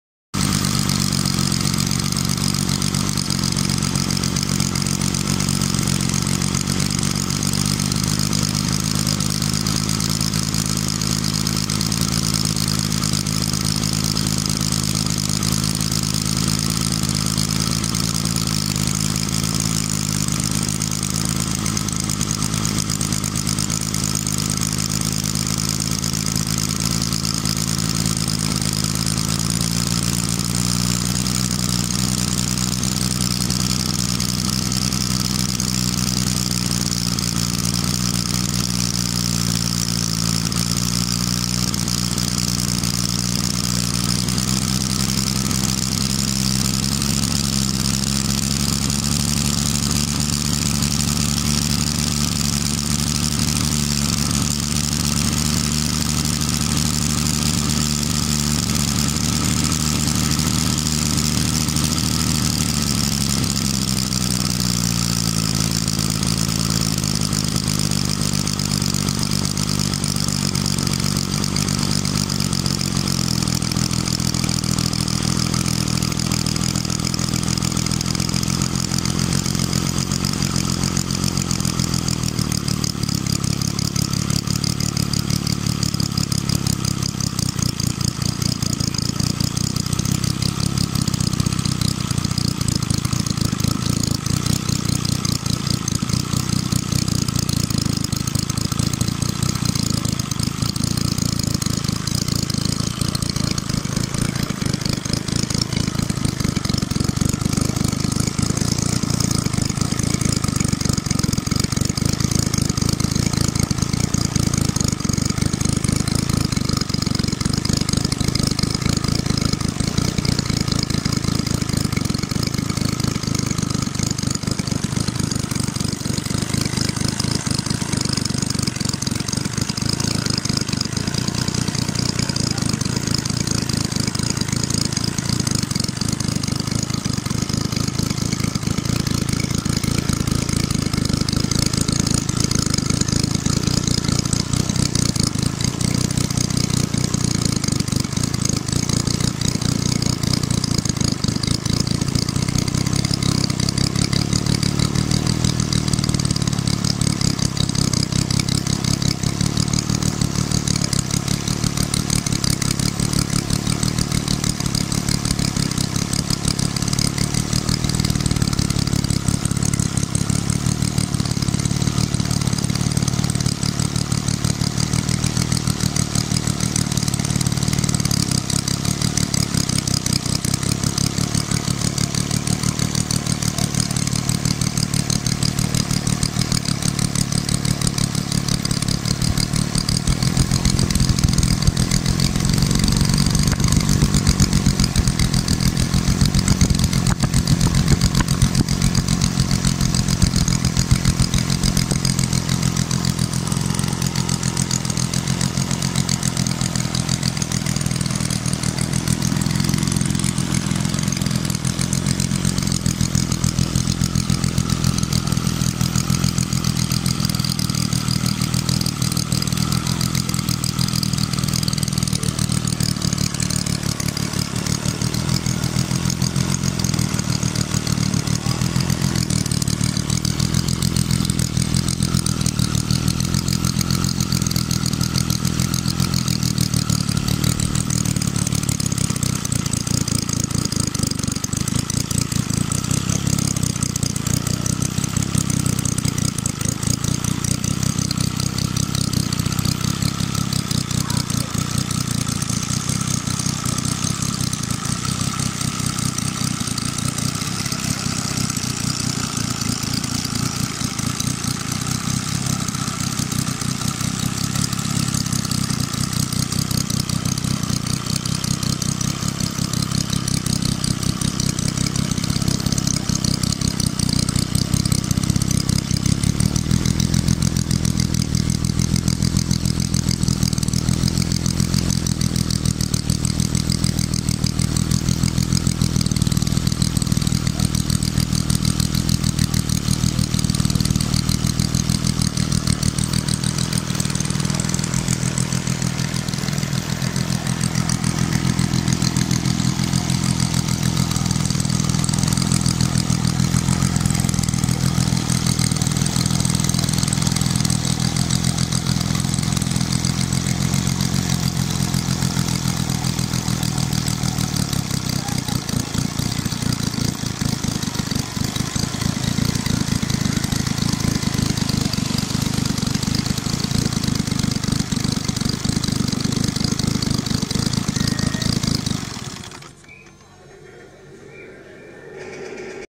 Modell-Schiffsdiesel läuft auch sehr langsam! -von Manfred König
Ich sage mir, genörgelt u.kritisiert ist schnell, aber selbst mal einen Mehrzylinder-Viertakt-Modellmotor konstruieren (nicht kopieren!!) und aus "Rohmaterial" alles selbst fertigen, (von der Kurbelwelle bis zur Steuerung...), daß fordert einem alles ab!!  Die besondere Kunst besteht darin, kleine Modell-Motoren zu extremen, stotterfreien Langsamlauf zu bringen!! Doch der Dreizylinder- Viertakter kann das!! Auch langsam sehr gleichmäßig zu laufen!! -Manfred König, der Erz Modellbauer.
 - nagging and criticising is quick, but building a model engine yourself (which runs!) is a different story! And I agree with many engine builders!  That is the art!  With small model engines, stutter-free long-running is often difficult to achieve. But the three-cylinder four-stroke can be regulated down very far! Take a look at the equally great video - "Super slow can the "model ship diesel without dampers"...A running report by Manfred König, the arch model builder.

pers"...A running report by Manfred König.